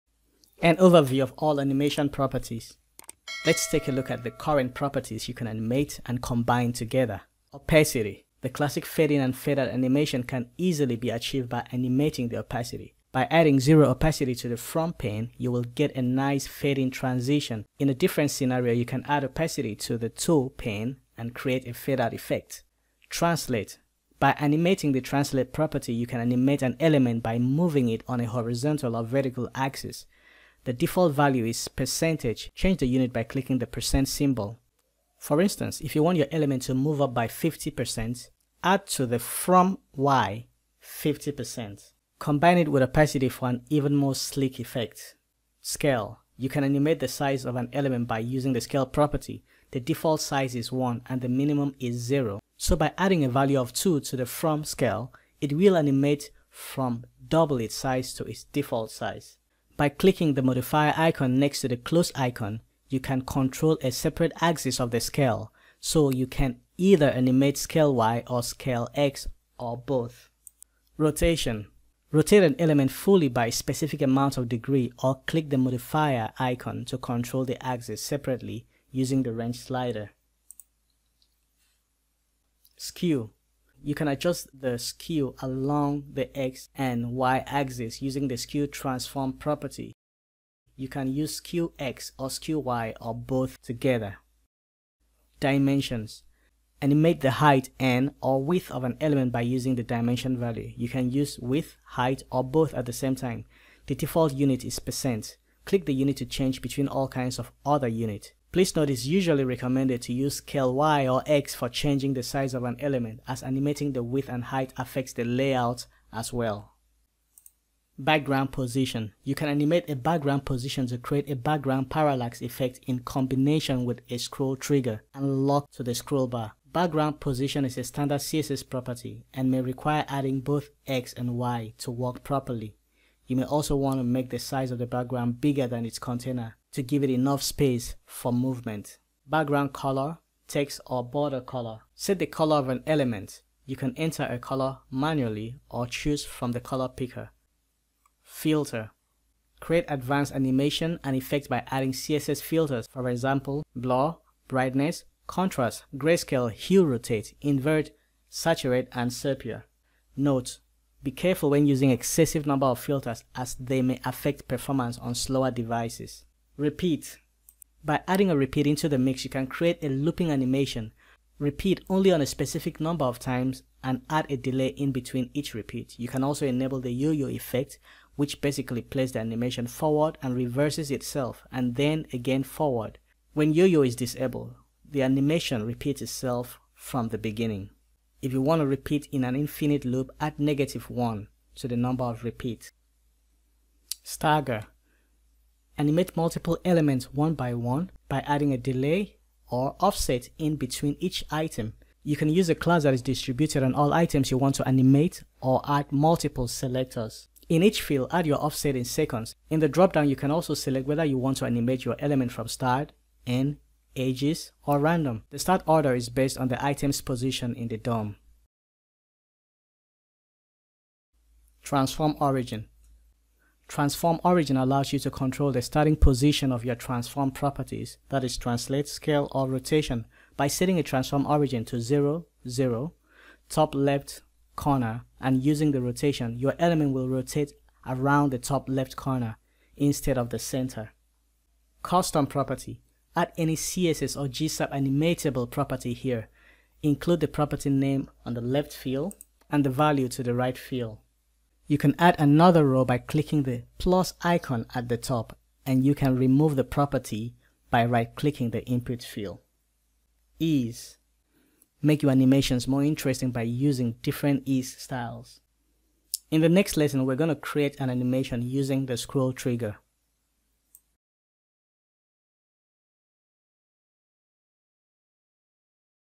PART 3 - GSAP animation properties in Motion Page for WordPress Websites
Beginners Guide to GSAP JavaScript Animation with motion page

This is the third part of a series to learn how to animate your WordPress website with GSAP animation using Motion Page, a graphical user interface animation builder.

Motion Page | WordPress | GSAP | Animation Tutorial